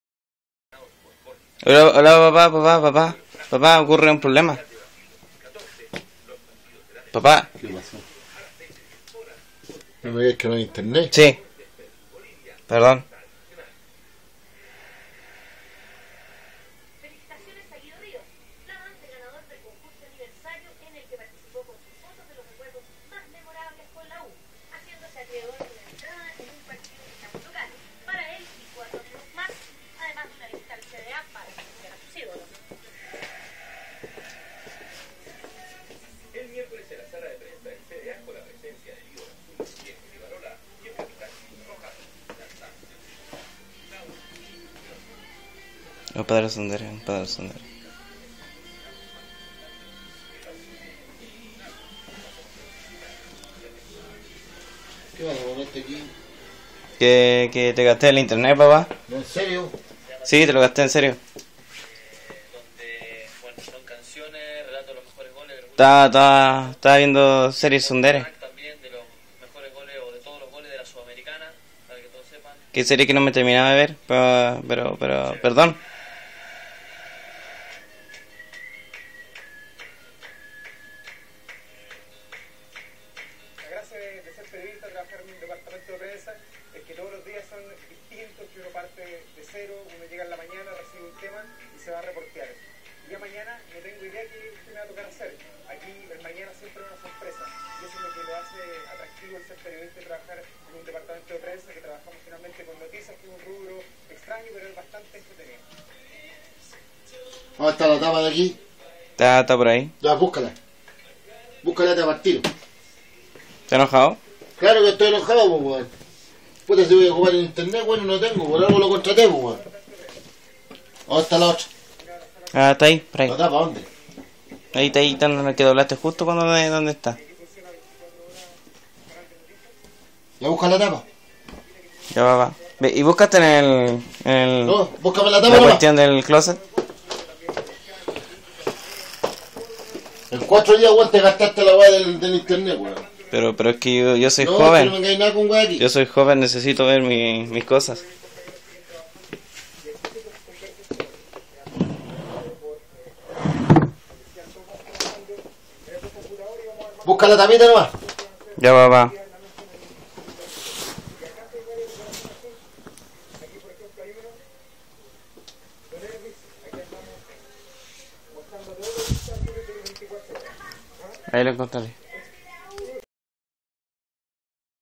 mi papa es tsundere y se enoja porque le gaste el interned
estaba viendo terminando de ver date a live y se le acabo el interned y mi apa se enojo deve ser por que no me quiere